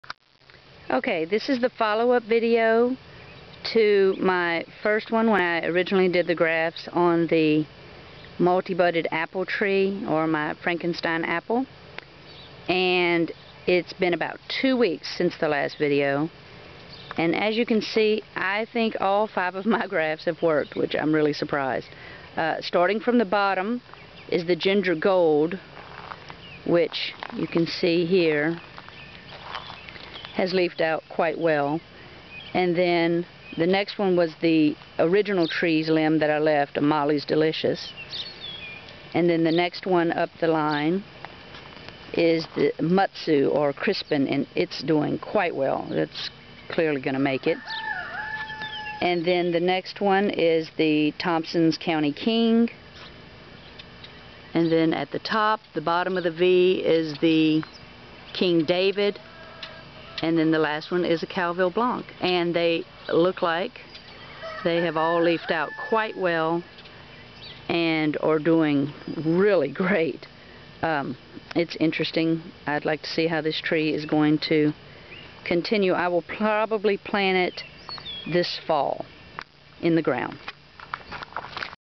Multi-grafted Apple Tree Project, Frankenstein, Multi-budded (Pt 2)
This is a Mollie's Delicious Apple tree, that I grafted 5 other apple varieties onto.
From the bottom: Ginger Gold, Mollie's Delicious (Original), Mutsu/Crispin, Tompkins County King, King David and Calville Blanc.
I plan to follow this tree as it grows.